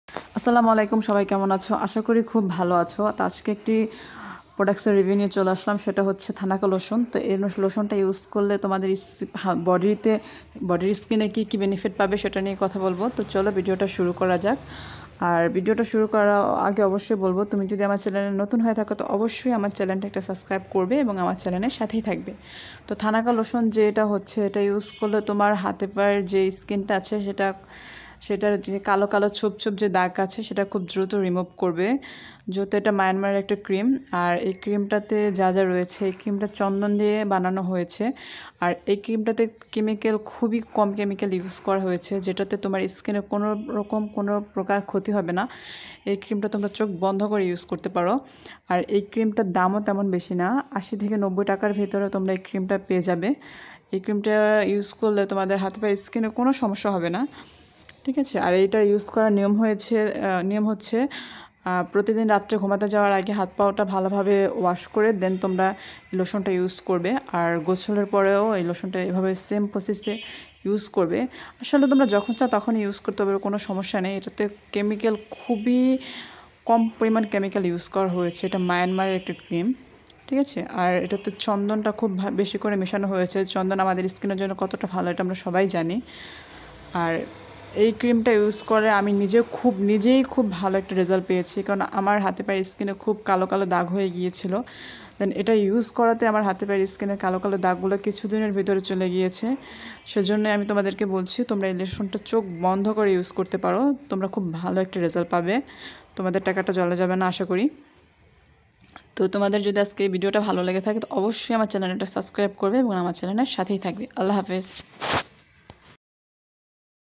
এই লোশনটা ইউজ করলে তোমাদের হাতে পায়ের স্কিনে কালো দাগ দূর হবে সপ্তাহখানেক এর ভেতর
এই ক্রিম টা ইউজ করলে তোমাদের হাতে পায়ের স্কিনে কালো ভাব দূর হবে তোমাদের স্ক্রিনকে ব্রাইট করবে এটা মিয়ানমারের একটি লোশন এই লোশনটা নাম হচ্ছে থানাকা লোশন খুবই ভালো একটা লোশন যেটা ইউজ করলে তোমরা খুব ভালো একটি রেজাল্ট পাবে এটাতে কোন প্রকার সাইড ইফেক্ট নেই